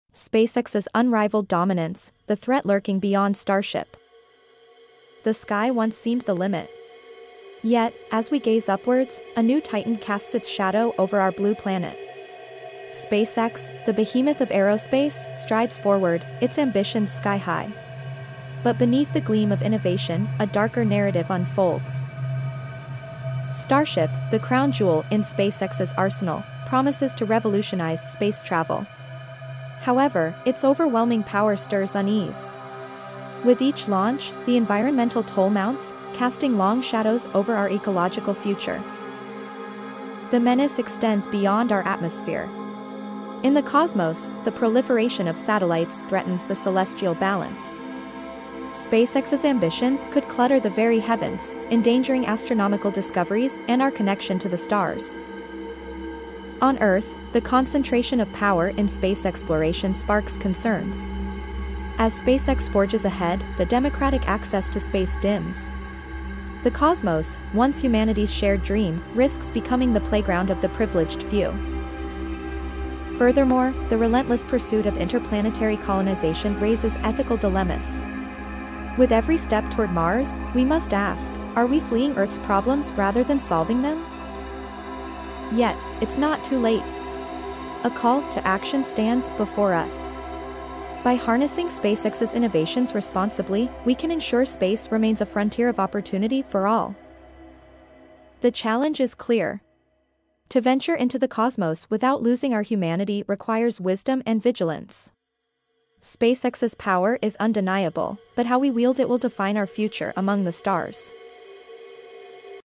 "SpaceX's Overwhelming Power: The Menace Beyond Starship"
**Description:**

In this highly captivating video, we delve into the awe-inspiring power of SpaceX and the extraordinary advancements beyond their exceptional Starship program. Prepare to be amazed as we explore the essence of SpaceX's groundbreaking achievements, showcasing their relentless commitment to space exploration.

Discover the unprecedented capabilities of SpaceX's innovative technologies, which enable mankind to reach new frontiers. We delve into the magnificent Starship, a vessel of immense power designed to revolutionize space travel and colonization. Witness the engineering marvels that fuel SpaceX's drive to conquer the final frontier.

Join us as we unveil the captivating stories of the brilliant minds behind SpaceX, their visionary leader Elon Musk, and the dedicated team propelling humanity towards a future among the stars. SpaceX's Overwhelming Power: The Menace Beyond Starship takes you on an exhilarating journey, revealing the revolutionary power that this aerospace giant wields.

Prepare to be astounded by stunning visuals, exhilarating launches, and groundbreaking achievements. Witness SpaceX's relentless drive to revolutionize space travel, pushing the boundaries of what is possible and inspiring generations to dream big and reach for the stars.

Tune in now to witness SpaceX's Overwhelming Power: The Menace Beyond Starship, a captivating exploration of humanity's quest to conquer the cosmos!